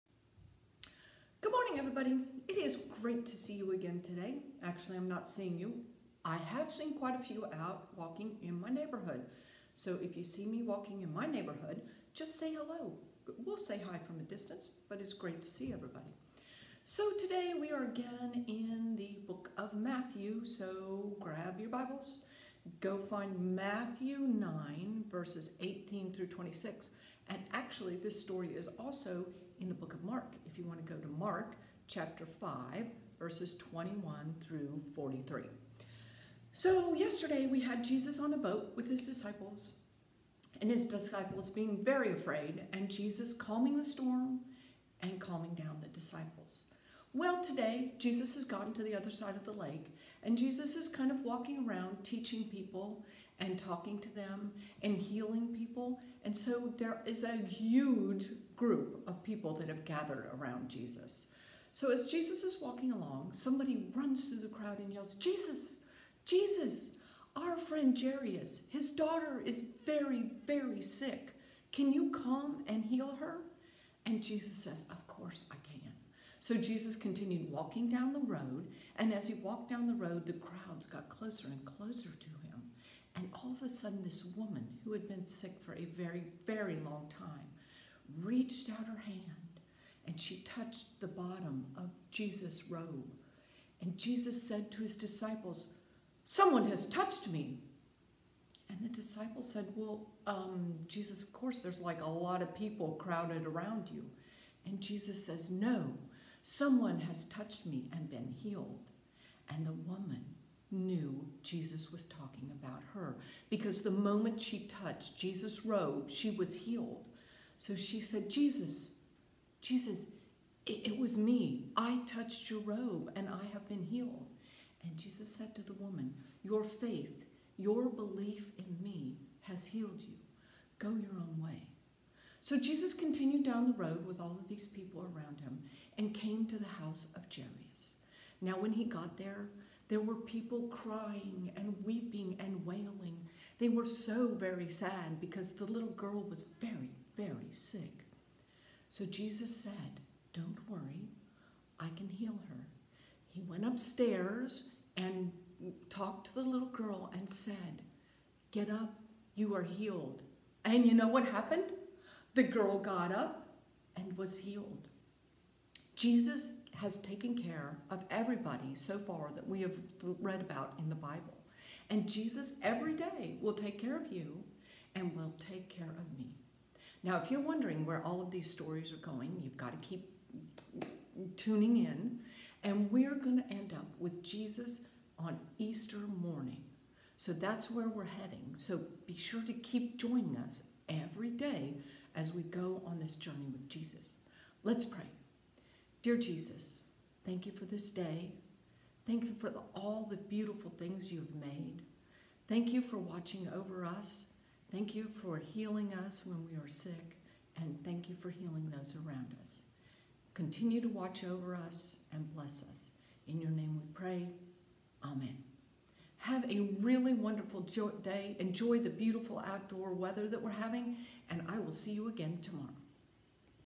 Virtual Children's Moment - Sunday, March 22
To keep connected to the kids in our congregation and community, our Children's Director, Sheri Lee, is recording videos to send out throughout the week to our email list and also to share on social media. We hope this helps the kids feel the love from their Church family and that they are still connected to everyone!

SCRIPTURE READING:
Matthew 9:18-26
International Children’s Bible (ICB)

Jesus Gives Life to a Girl and Heals a Sick Woman
18 While Jesus was saying these things, a ruler of the synagogue came to him. The ruler bowed down before Jesus and said, “My daughter has just died. But come and touch her with your hand, and she will live again.”

19 So Jesus stood up and went with the ruler. Jesus’ followers went too.

20 Then a woman who had been bleeding for 12 years came behind Jesus and touched the edge of his coat. 21 She was thinking, “If I can touch his coat, then I will be healed.”

22 Jesus turned and saw the woman. He said, “Be happy, dear woman. You are made well because you believed.” And the woman was healed at once.

23 Jesus continued along with the ruler and went into the ruler’s house. Jesus saw people there who play music for funerals. And he saw many people there crying. 24 Jesus said, “Go away. The girl is not dead. She is only asleep.” But the people laughed at Jesus. 25 After the crowd had been put outside, Jesus went into the girl’s room. He took her hand, and she stood up. 26 The news about this spread all around the area.